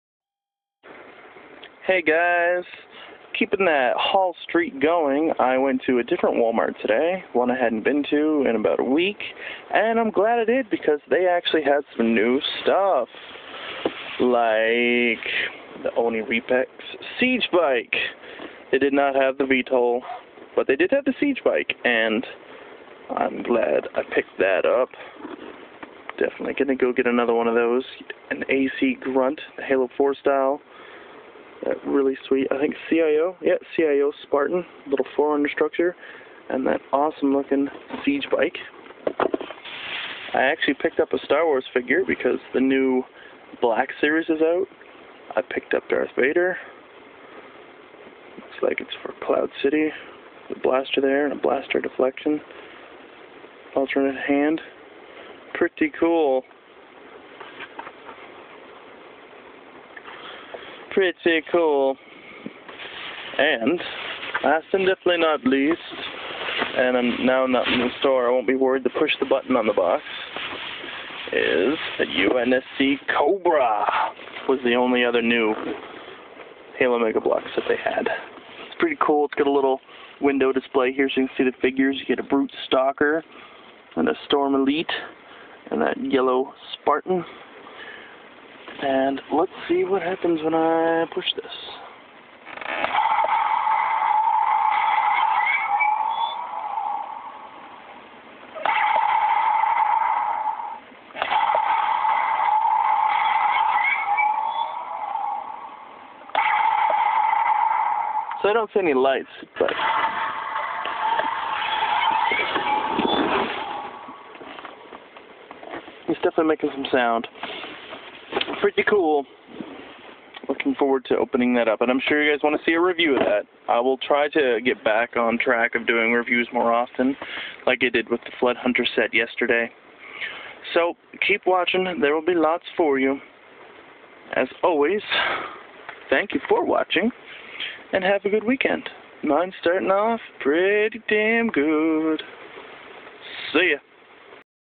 New Halo Mega Bloks! Cobra! Siege Bike! Haul! Darth Vader!
Went to Wal-Mart today and was lucky enough to find two new Halo sets! The UNSC Cobra and the ONI Reap-X Siege Bike! Plus they had a ton of the new Black Series Star Wars figures! So I picked up the wicked looking Darth Vader! Review coming soon for the Cobra and Siege Bike!